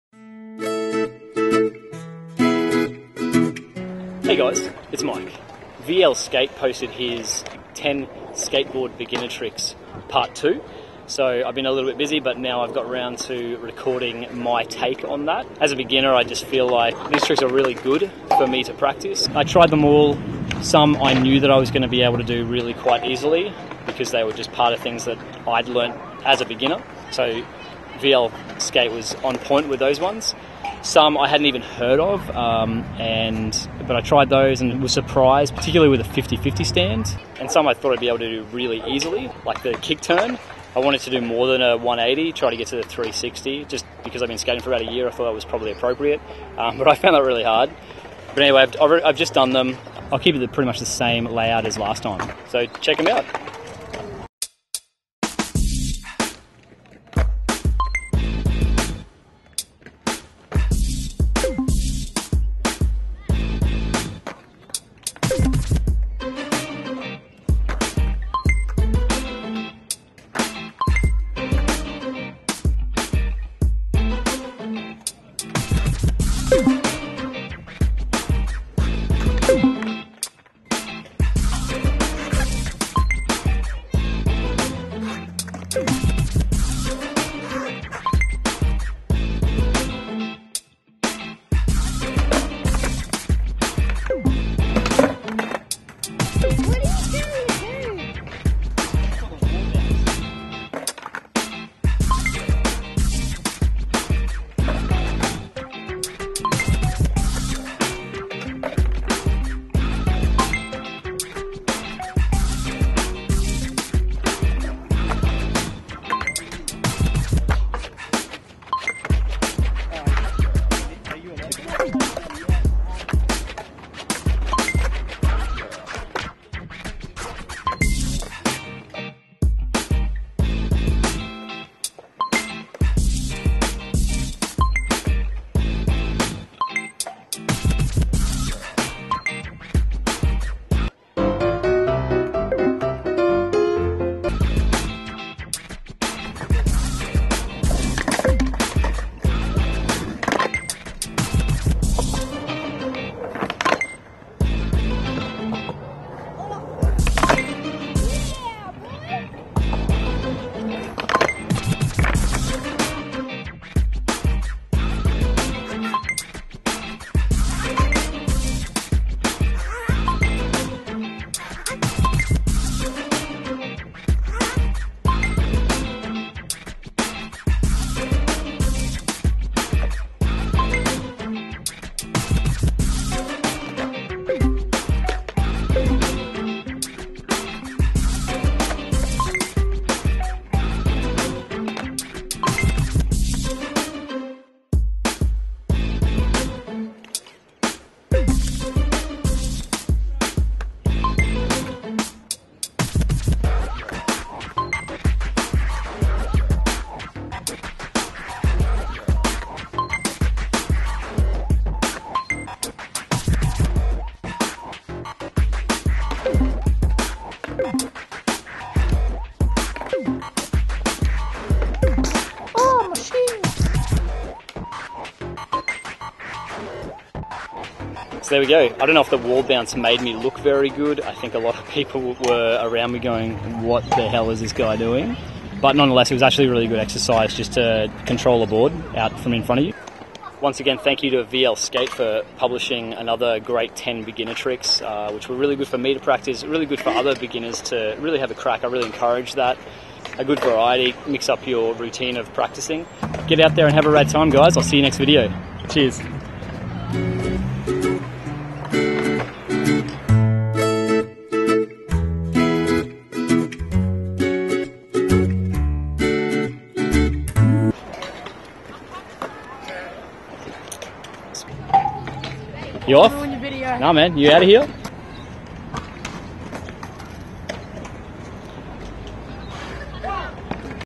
10 Easy Beginner Skateboard Tricks Part 2 | VLSkate Challenge
VLSkate set them up and tried them again. I felt a little more comfortable this time but I think these challenges are a great way to indroduce more variety into your skate practice. I really enjoyed the Firecracker and kept trying the Wall Bounce for ages after but will probably never do it again lol.

Trick List:
Bomb drop / Acid Drop / Caveman (so many conflicting names on YouTube but haters are gonna hate ;P)
Powerslide
Wall Bounce
Poor Man's Nose Stall
Manual
Firecracker
Backside 180 Revert
50 50 Stand
Kickturn
Kick up Pick up